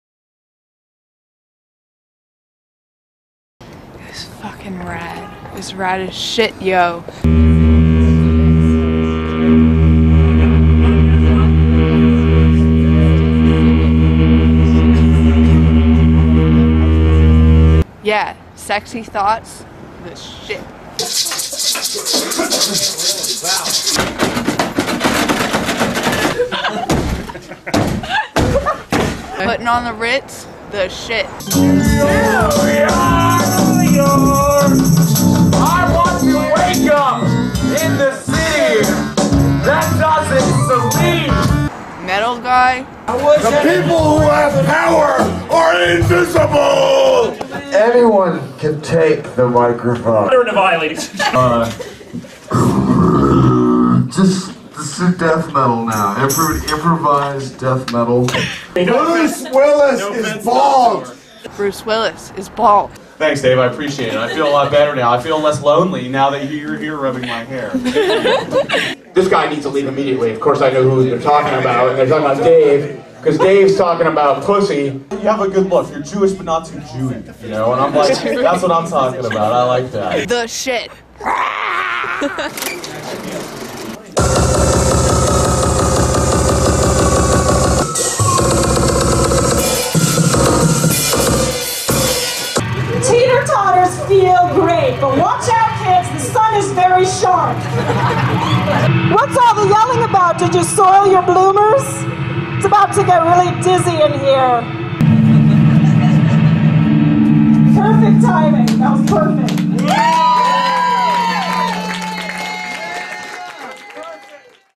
Wednesday Night is Experimental Jazz!
Erin and Teresa go to a sick show in Williamsburg.